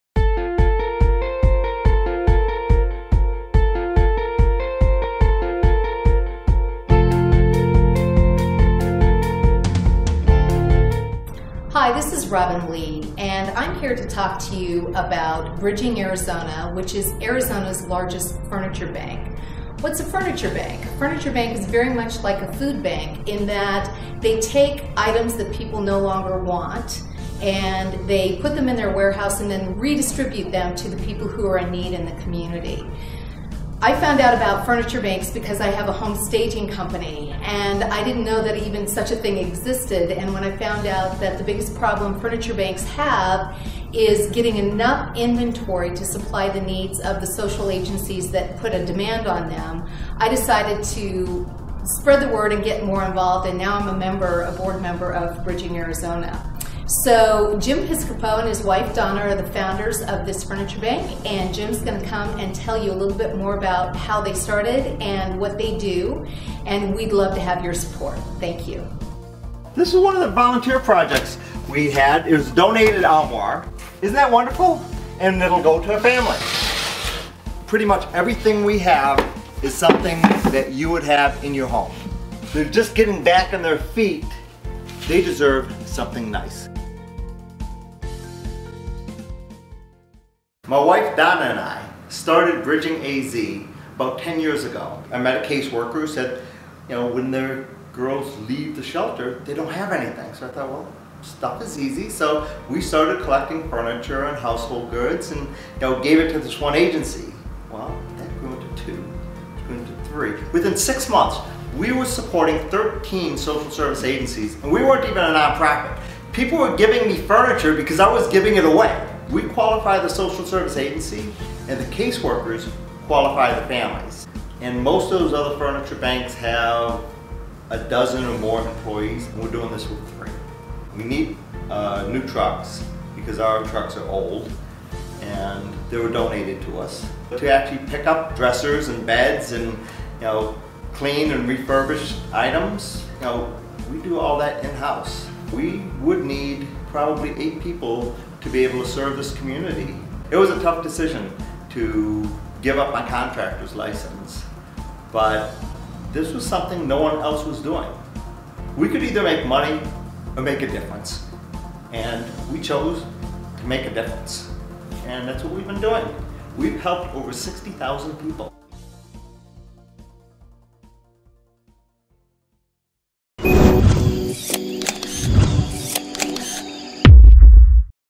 Bridging AZ Furniture Bank
Bridging AZ is a furniture bank based in Mesa, Arizona but services the entire valley. Started by Jim Piscopo and his wife Donna in 2004, Bridging AZ Furniture Bank is a non-profit organization that provides families in need with the furniture and household items necessary to create a safe, comfortable and healthy home environment. Please make a tax deductible donation today and help Bridging AZ with it's mission of providing basic furniture and household goods to economically disadvantaged individuals and families living in our community. THANK YOU!!